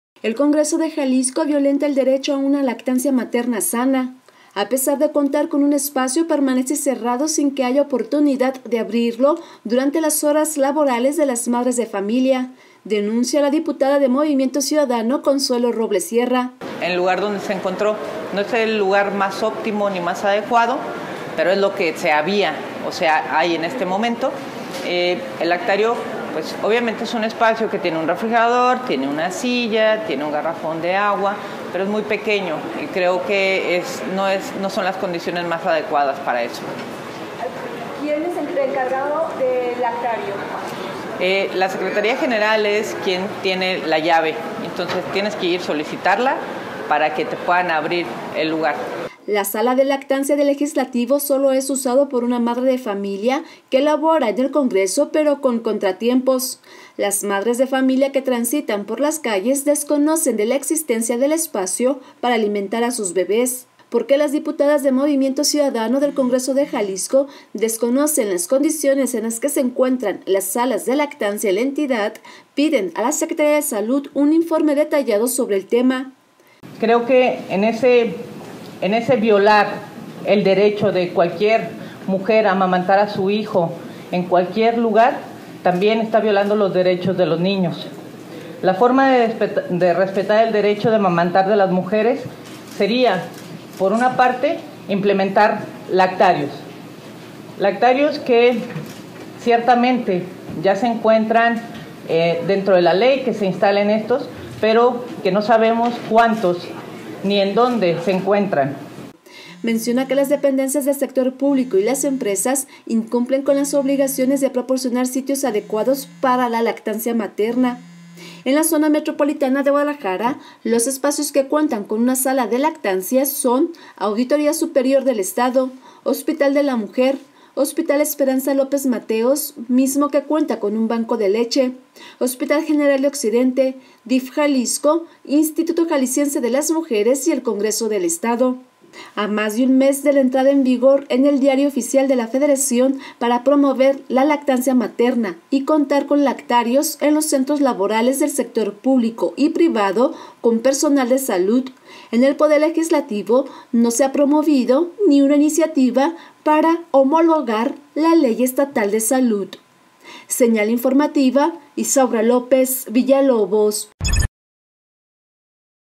Señal Informativa: Congreso de Jalisco violenta derecho a lactancia materna sana, denuncia diputada
A pesar de contar con un espacio permanece cerrado sin que haya oportunidad de abrirlo durante las horas laborables de las madres de familia, denuncia la diputada de Movimiento Ciudadano, Consuelo Robles Sierra.